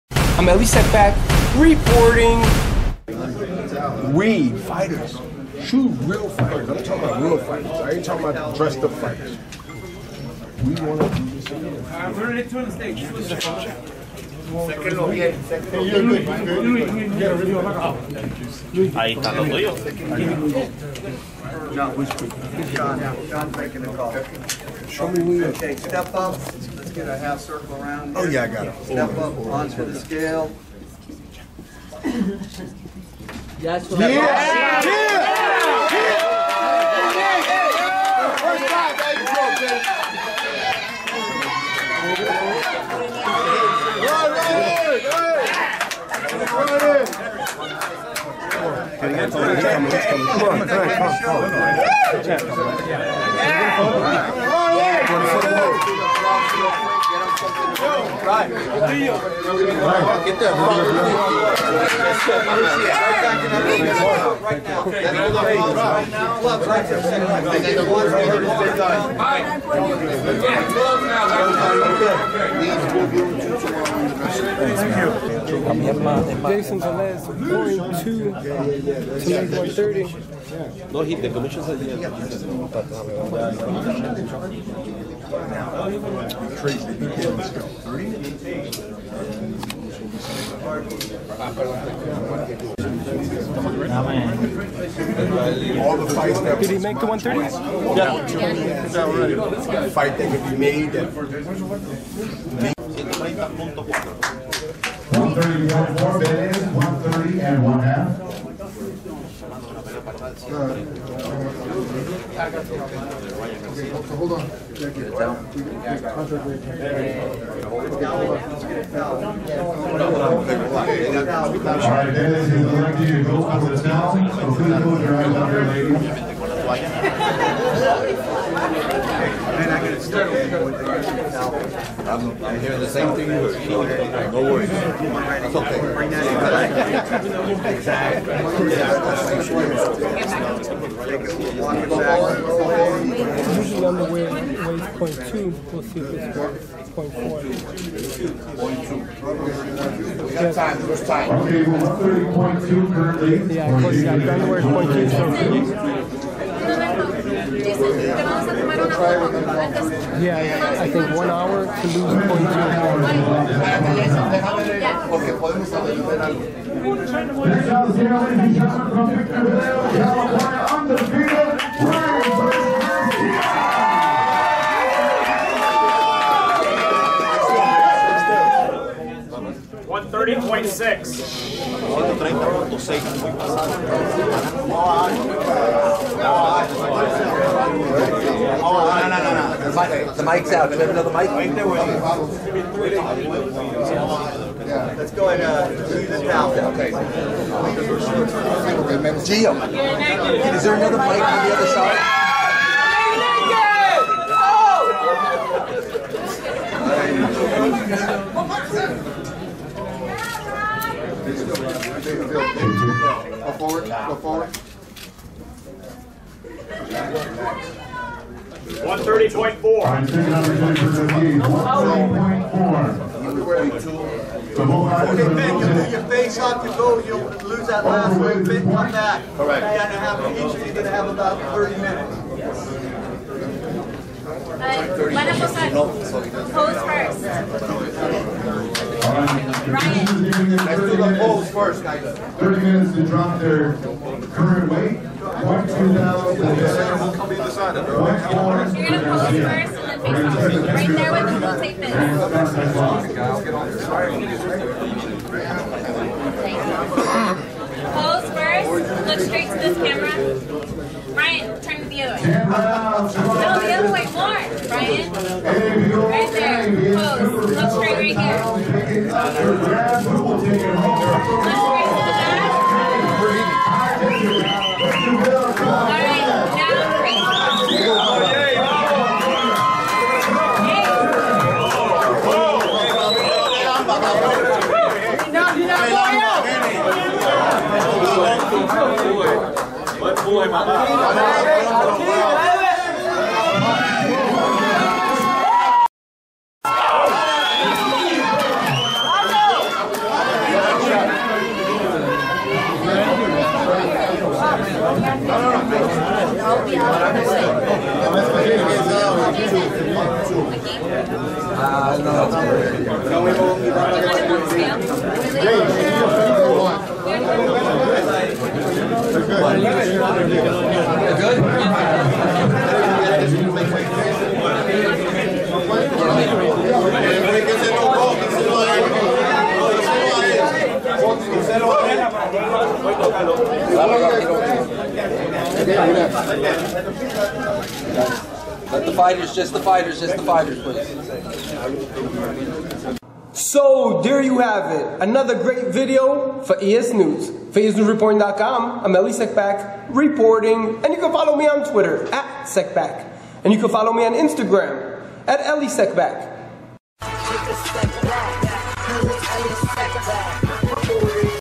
(MUST SEE) Ryan Garcia Erupts After Making 130 Ready For Jayson Velez EsNews Boxing
(MUST SEE) Ryan Garcia Erupts After Making 130 Ready For Jayson Velez